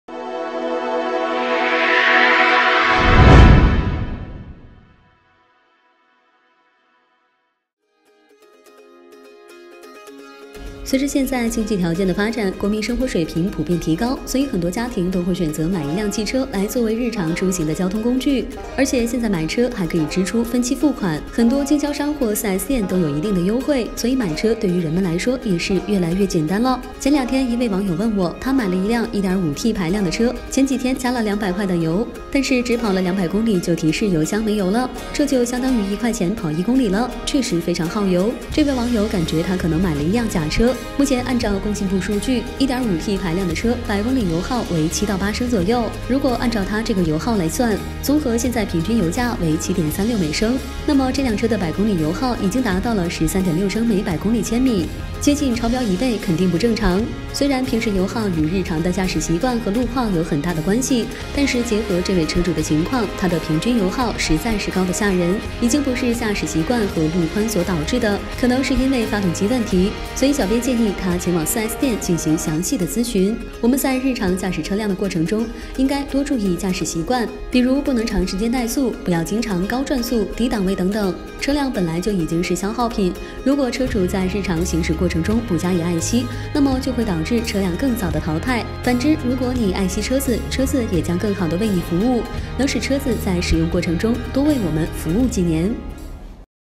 1.5T的車子，200塊錢的油只跑了200公裏，我是買了假車嗎？
隨著現在經濟條件的發展，國民生活水平普遍提高，所以很多家庭都會選擇買一輛汽車來作為日常出行的交通工具，而且現在買車還可以支出分期付款，很多經銷商或4S店都有一定優惠，所以買車對於人們來說也是越來越簡單了。